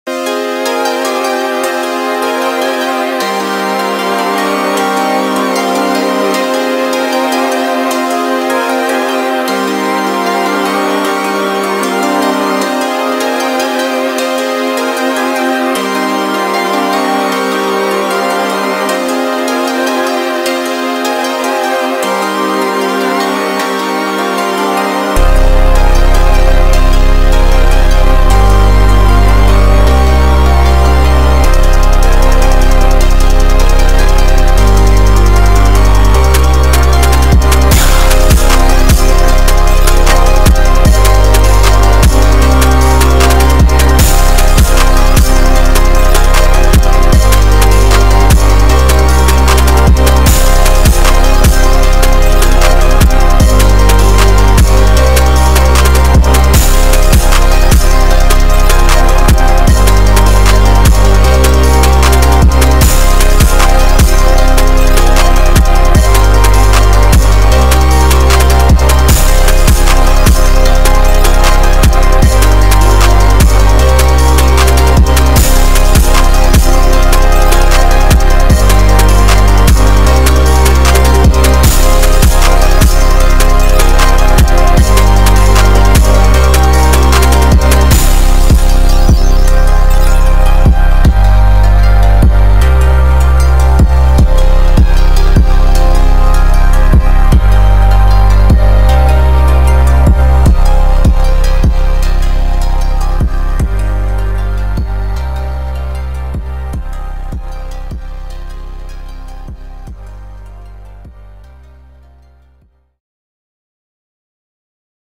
*free* axxturel x bacleo x sigilkore type beat
experimental af ion know wtf im doin

153 bpm

f min

free for non profit, must use prod. niuru in the title.